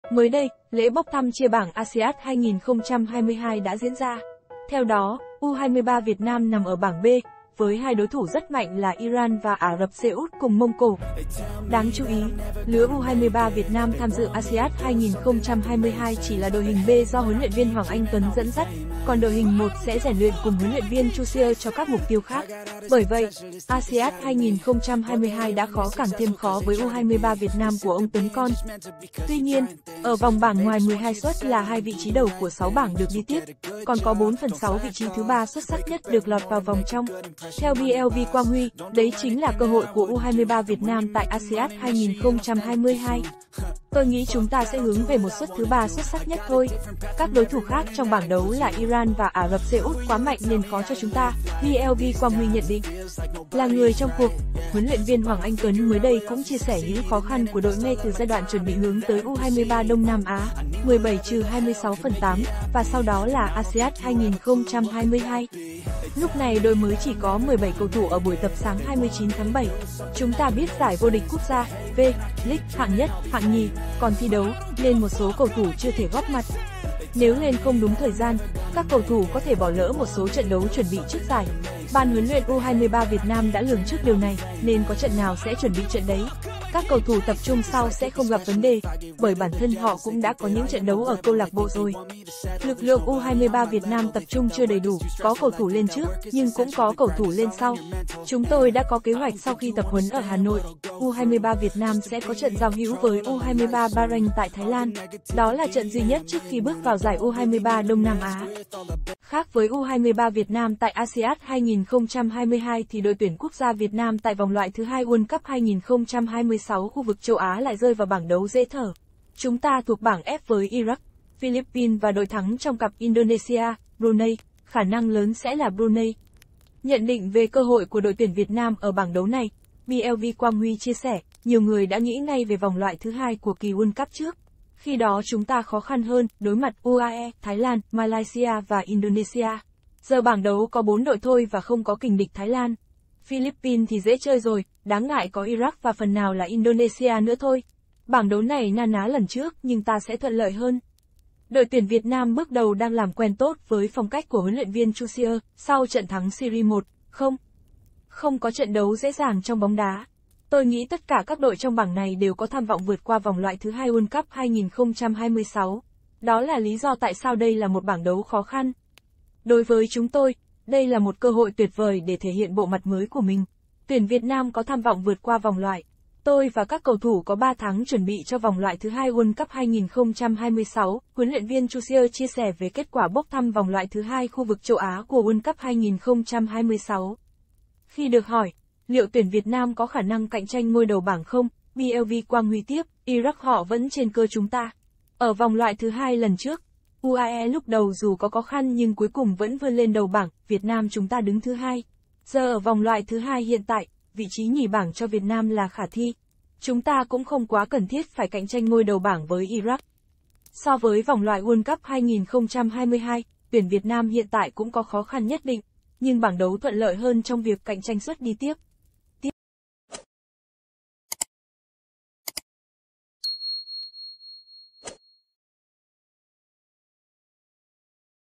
U23 VIỆT NAM TẠI ASIAD 2022 | CƠ HỘI VƯỢT QUA VÒNG BẢNG KHÓ KHĂN
Đáng chú ý, lứa U23 Việt Nam tham dự Asiad 2022 chỉ là đội hình B do  Huấn luyện viên Hoàng Anh Tuấn dẫn dắt, còn đội hình một sẽ rèn luyện cùng  Huấn luyện viên Troussier cho các mục tiêu khác. Bởi vậy, Asiad 2022 đã khó càng thêm khó với U23 Việt Nam của ông Tuấn con.